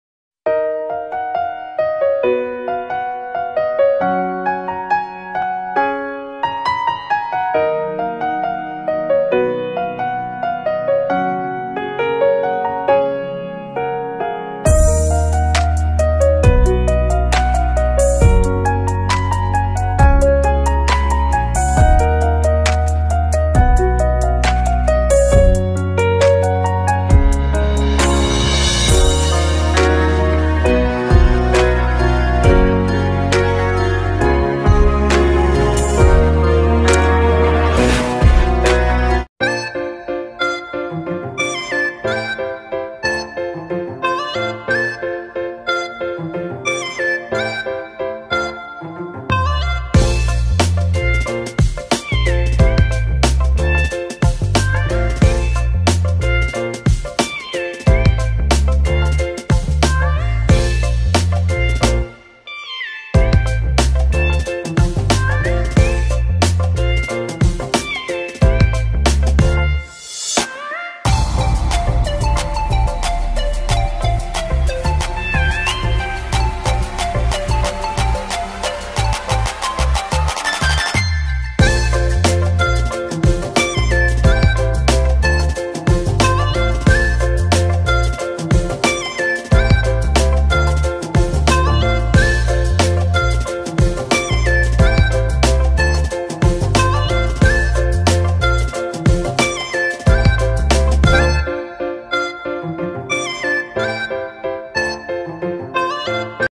Ruang Tamu Minimalis//Dekorasi Rumah Minimalis
Assalamualaikum warahmatullahi wwb...
Dalam video ini kami menampilkan ruang tamu minimalis, semoga menjadi inspirasi kita semua.